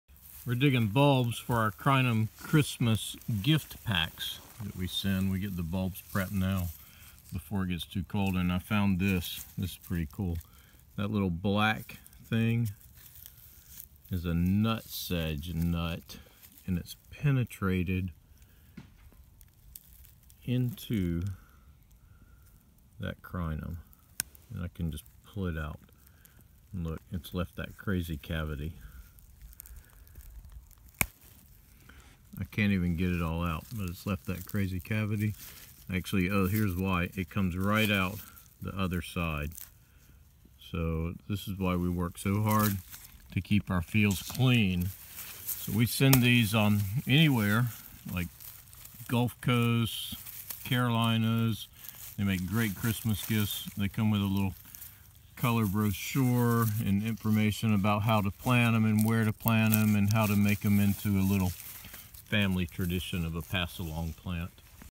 crinum bulb with nutsedge growing through it.
Crinum lily bulbs freshly dug. One bulb has a Nutsedge root and nutlet growing through the neck of the Crinum bulb.